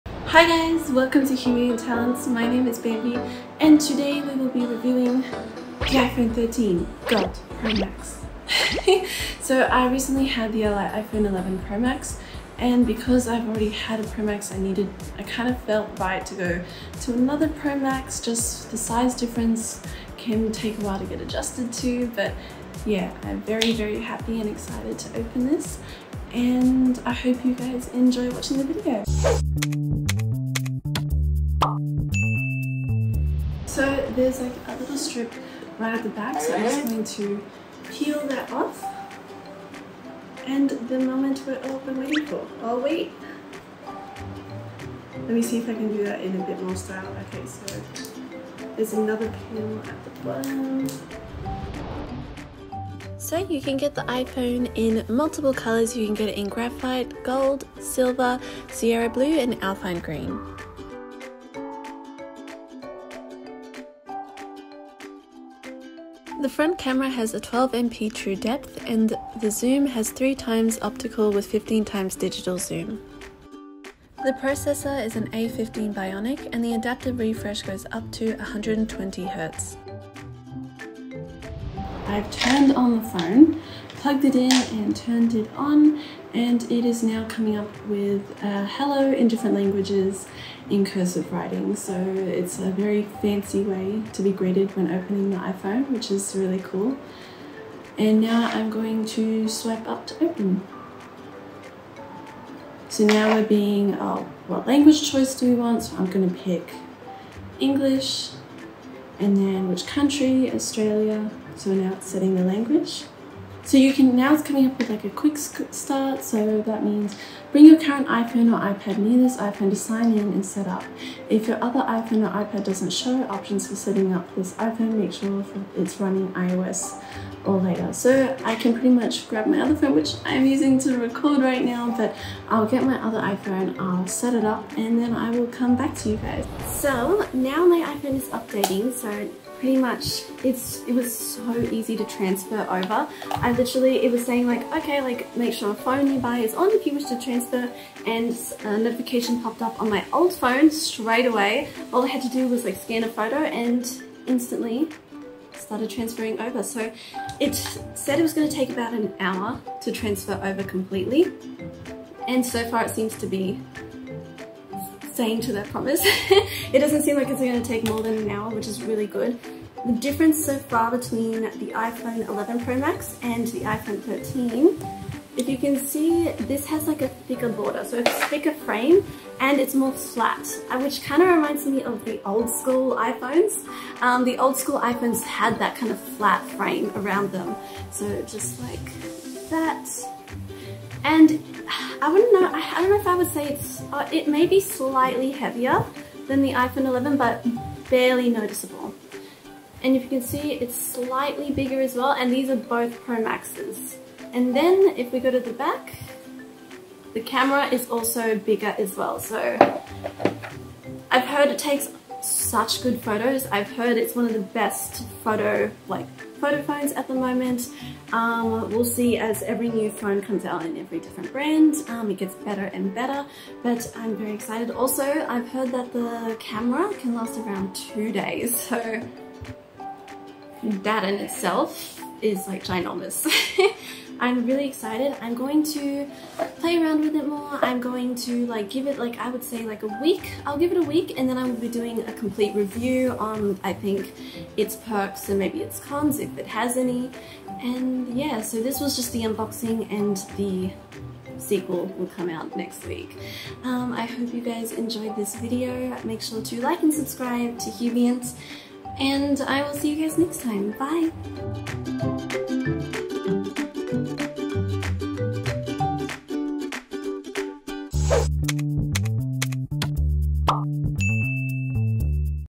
Unboxing iPhone 13 Pro Max | Skybambie
Watch Sky Bambie unbox her new iPhone 13 Pro Max! Enjoy!

Follow HUBient and join us for more exciting things to come!